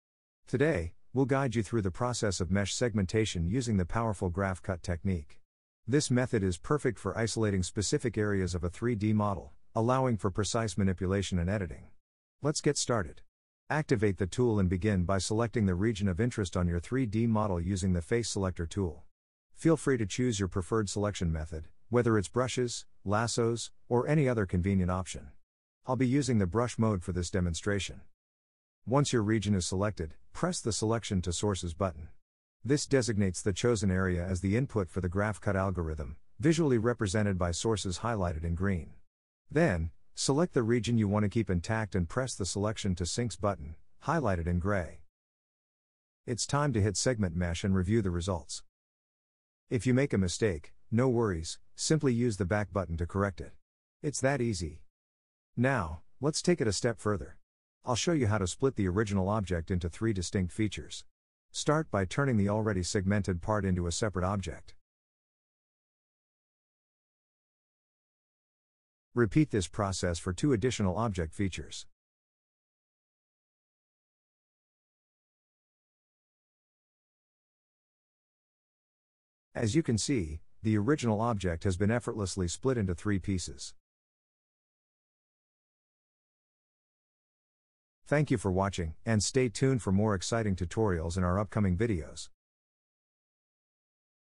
How to Segment Mesh using Graph Cuts
Learn how to precisely isolate areas of your 3D model for easy manipulation and editing using the Graph Cut technique! In this tutorial, we will guide you through the process—select your region of interest, designate sources and sinks, and effortlessly segment your mesh. Master splitting your original object into distinct features with MeshInspector. It's that easy!